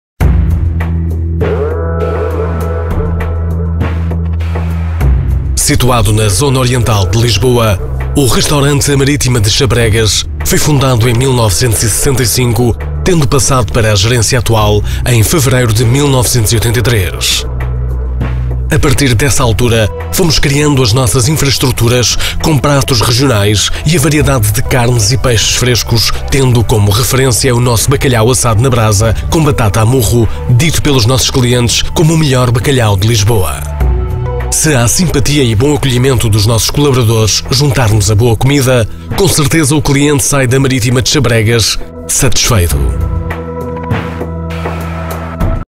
Restaurante A Marítima de Xabregas
Situado na zona oriental de Lisboa A Marítima de Xabregas foi fundado em 1965. Temos uma varieda de carnes e peixes fresco à sua disposição.